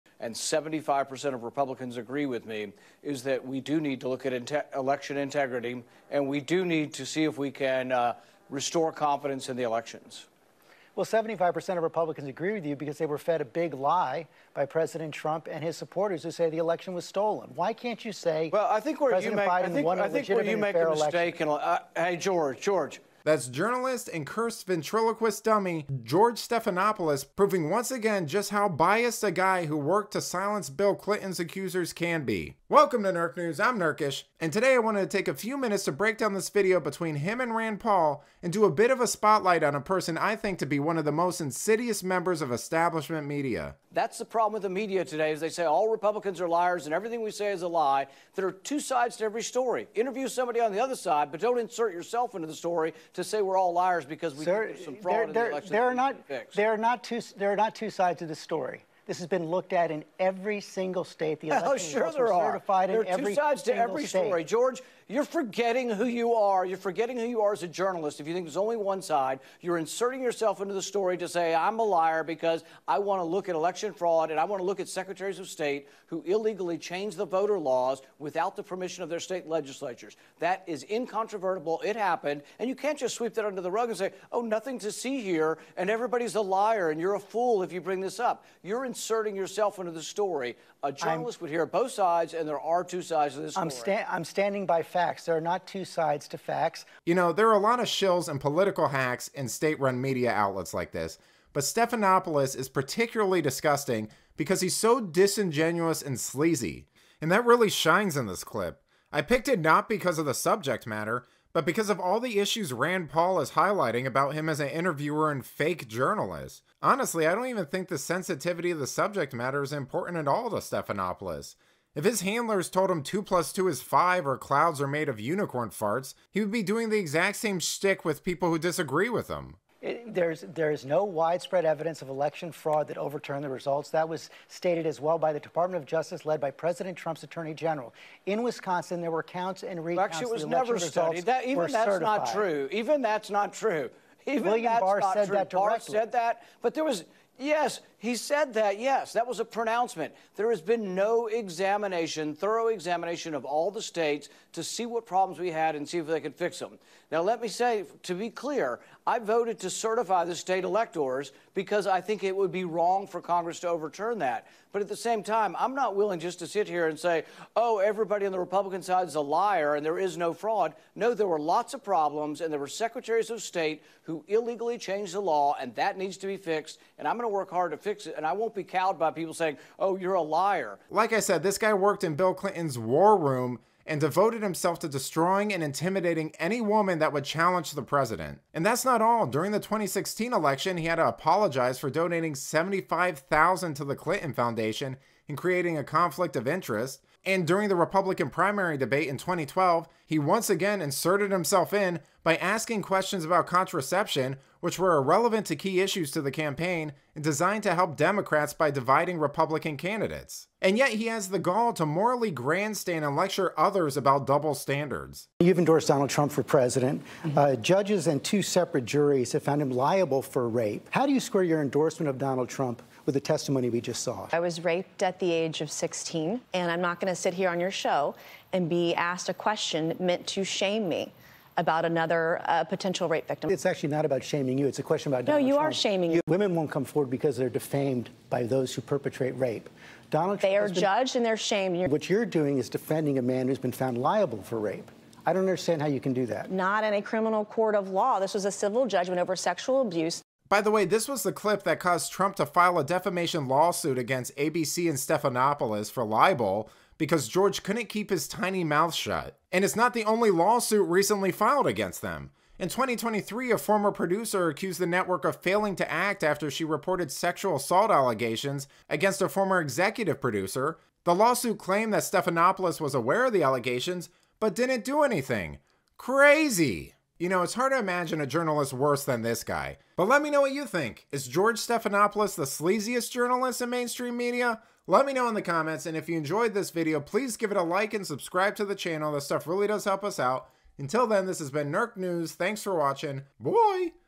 Leftist Host Tries To TRAP Rand Paul INSTANTLY Regrets It
Watch as a leftist host tries to trap Rand Paul, only to instantly regret it! This interview is a must-watch for anyone interested in political debates and discussions.